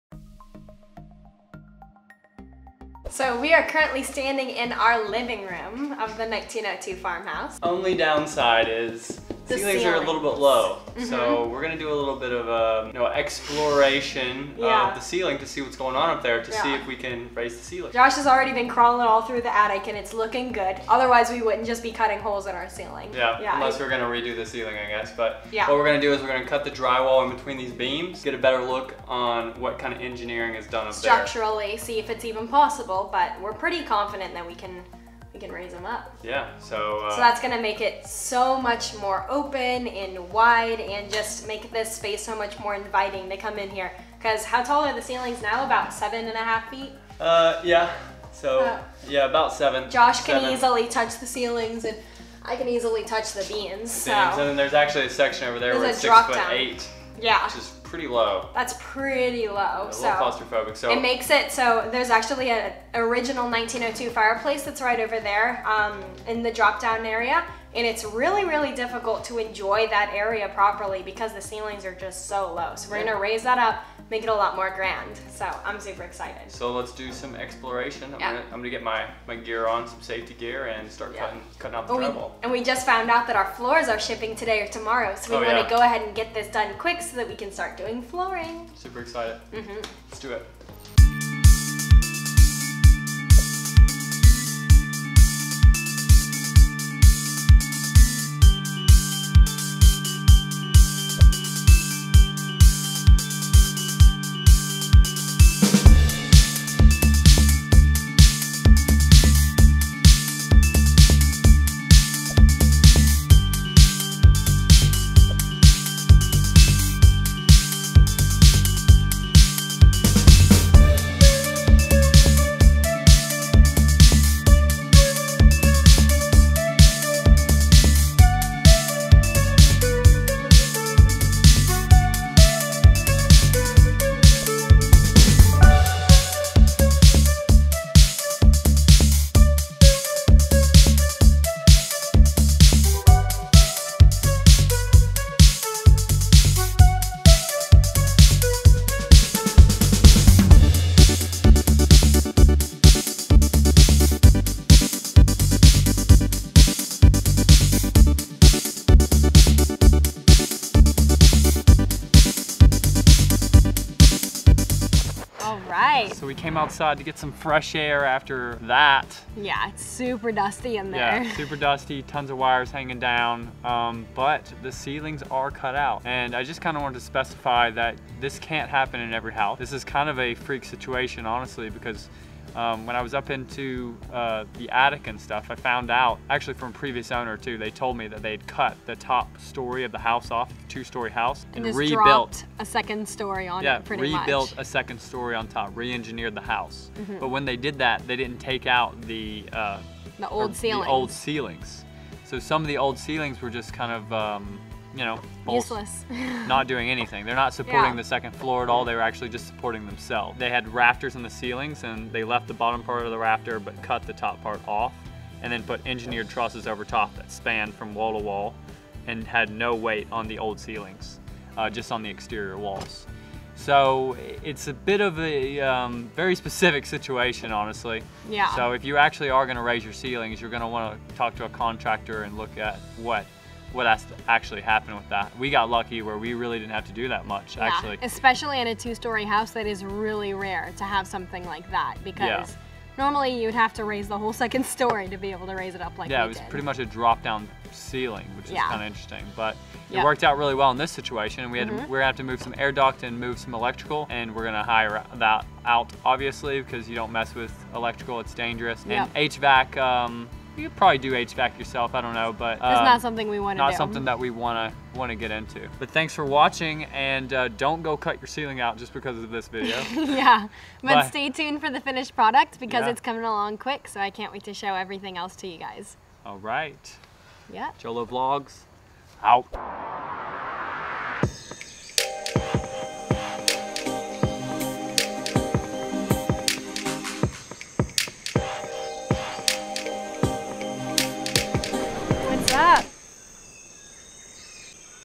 Tearing out our living room ceiling
In this video, we tear out our living room ceilings. Its quite a mess and you won't believe what it looks like under all that drywall. If you want to raise your ceilings please consult a contractor before tearing everything out.